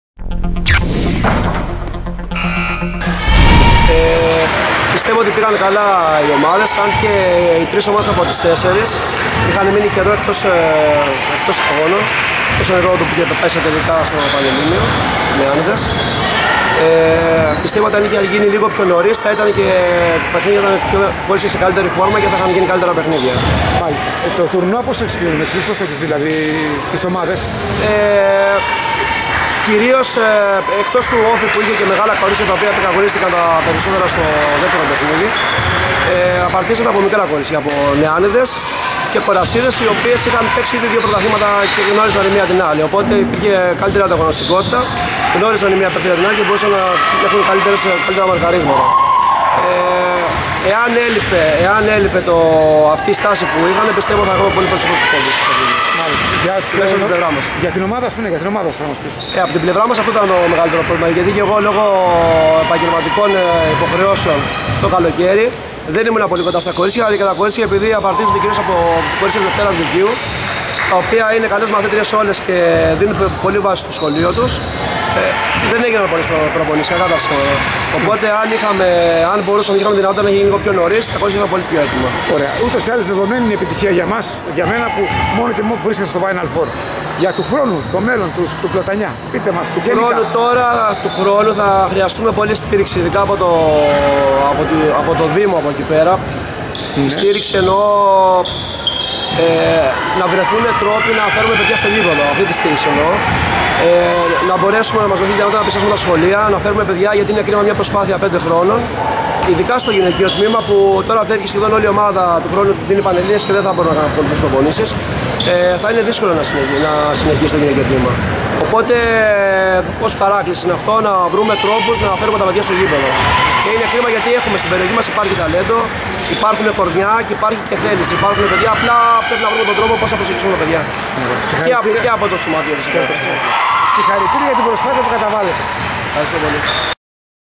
Δηλώσεις Μάνου Σταματάκη , Πλατανιά
Ο προπονητής του Πλατανιά κ.Μάνος Σταματάκης,  μίλησε στο kritikobasket.gr, για την παρουσία της ομάδας του στο final four Γυναικών ΕΚΑΣΚ  και γενικά για την ομάδα του Ταυρωνίτη.